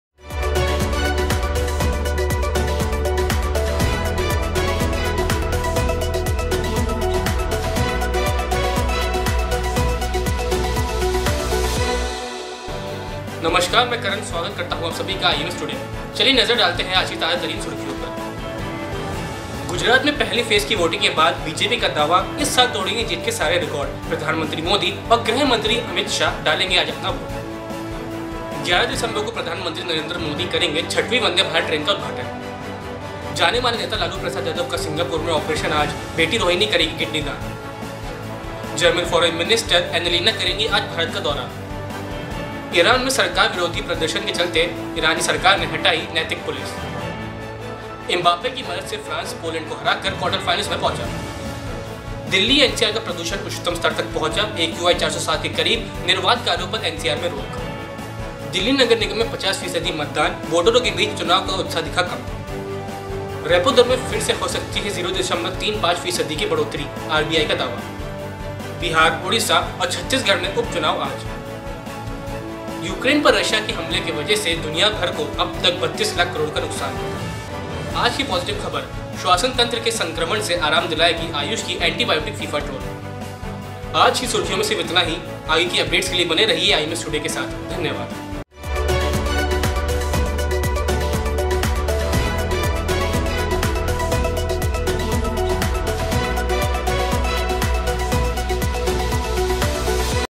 NEWS HEADLINES || IMS TODAY
1. पीएम मोदी और अमित शाह आज डालेंगे वोट ।
2. पीएम करेंगे वंदे भारत ट्रेन का उद्घाटन ।
3. जाने माने  नेता लालू प्रसाद यादव का आज ऑपरेशन।
4. जर्मन फॉरेन मिनिस्टर आज करेगी भारत का दौरा ।
5. ईरान में सरकार को हटानी पड़ी नैतिक पुलिस ।
6. फ्रांस पहुंचा क्वार्टर फ़ाइनल में।
7. दिल्ली एनसीआर में बड़ा प्रदूषण ।
8. दिल्ली नगर निगम में हुए 50 फ़ीसदी मतदान ।
9. रेपो रेट में 0.35% बढ़ोतरी।
10. बिहार उड़ीसा छत्तीसगढ़ में आज चुनाव।
11.रशिया यूक्रेन वर के चलते दुनिया को अब तक 32 करोड का नुकसान।
12.श्वसन तंत्र के संक्रमण से मिलेगा आराम ।

Camera: ATUL SRIVASTAVA
Editing: ATUL SRIVASTAVA
Support: SIMRAN, SHREEYA